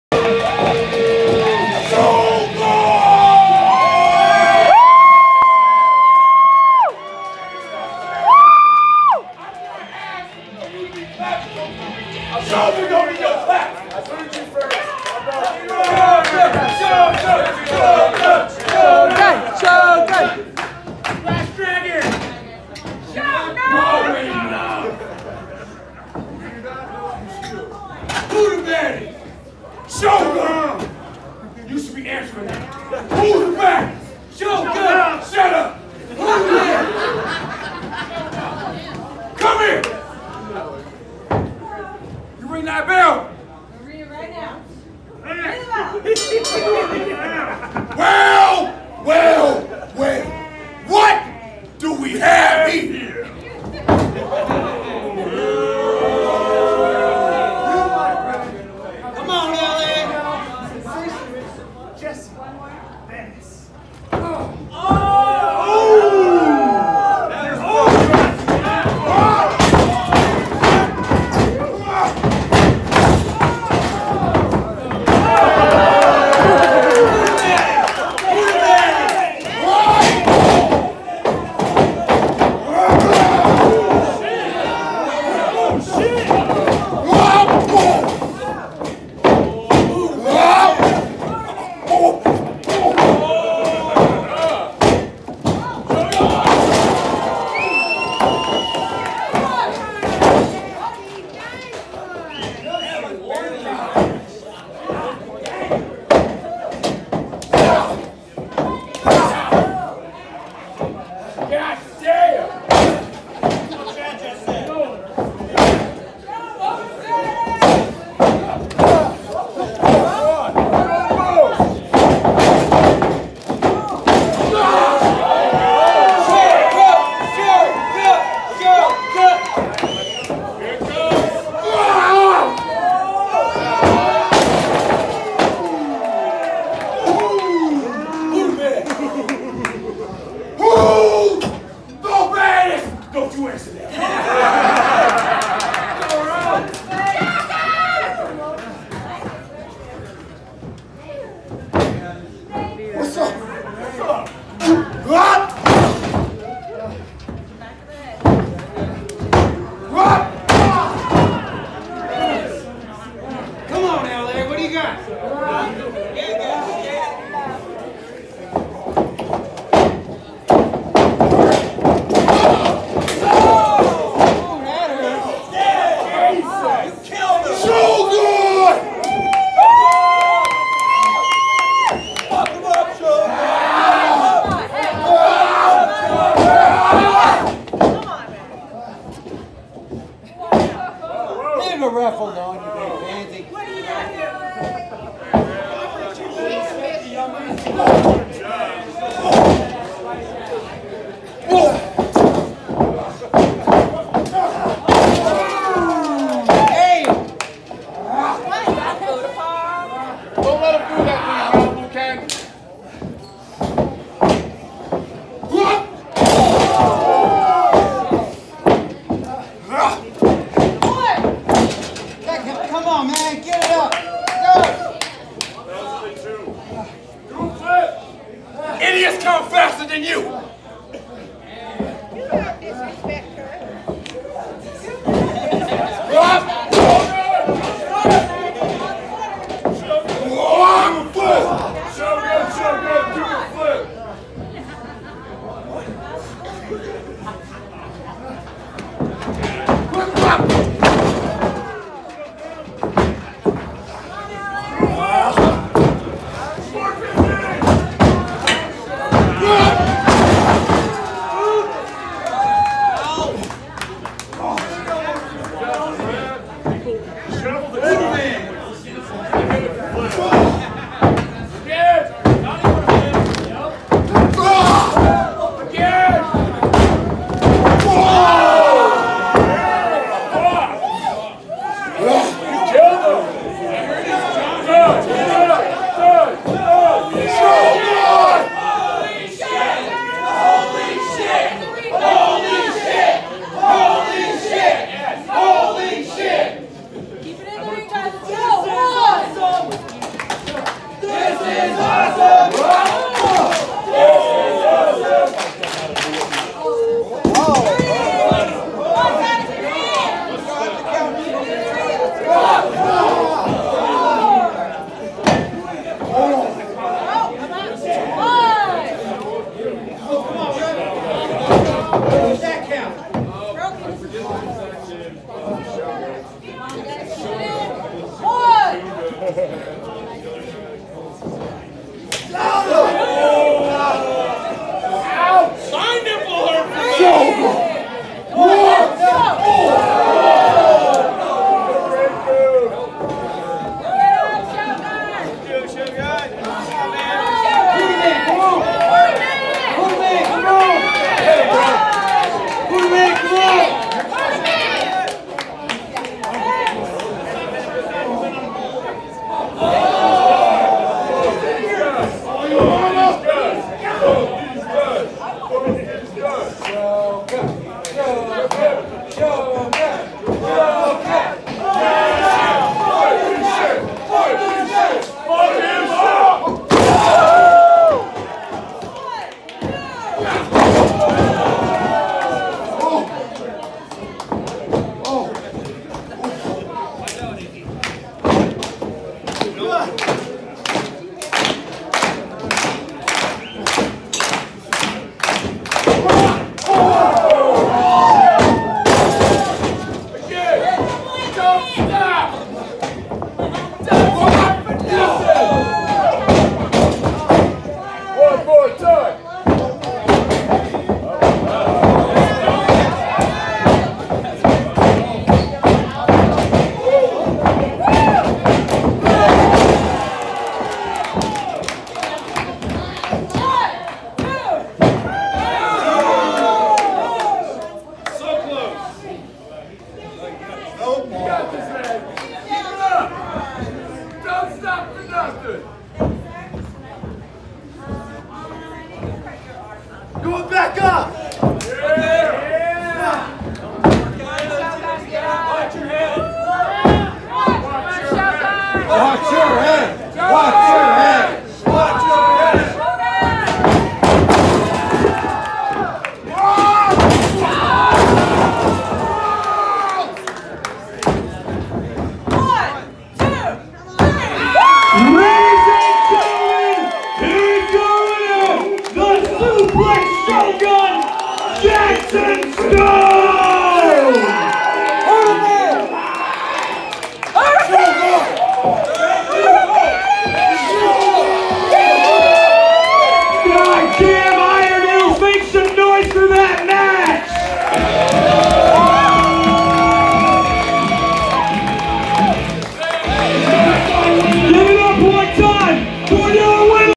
“The Suplex Shōgun” Jackson Stone vs. “The SO Cal Sensation” Jesse Venice
Shōgun travels to the mountains of Colorado Springs, Colorado as he makes his debut not only in CO but also in IRON HILL CHAMPIONSHIP WRESTLING!

His opponent for this evening has as high flying abilities to match his style! Tony Venice!

Check out Iron Hill Championship Wrestling

And follow Jackson Stone 👺🍋
Facebook: Jackson Stone (Athlete Page)